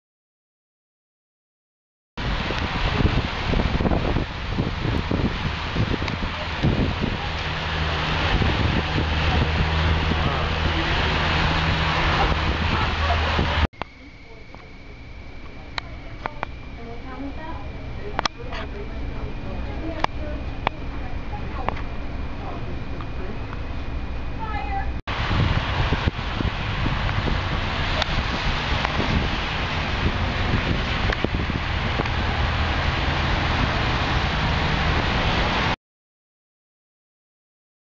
Fire from Lightning Storm - Nashua NH
Lightning brought down power line that caused a tree to catch fire on Amherst St in Nashua.  ~5:45pm o n May 4. 2010